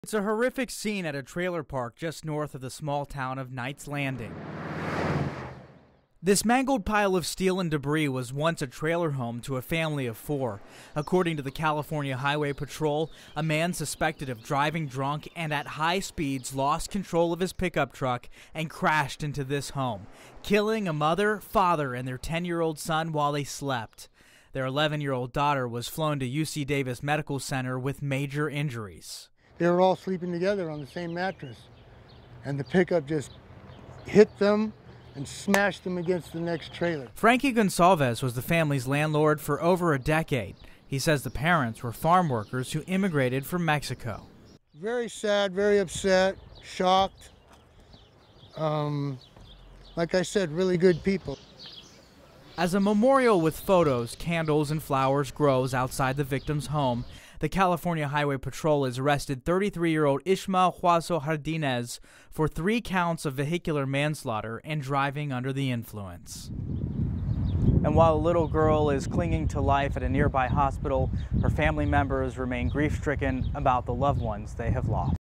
Alleged DUI driver crashes into trailer killing 3, injuring 1 girl in Knights Landing
Ismael Huazo-Jardinez was arrested on charges of vehicular manslaughter and felony DUI. Authorities say a family of four was struck by the vehicle, killing 3 and injuring one 11 year-old-girl.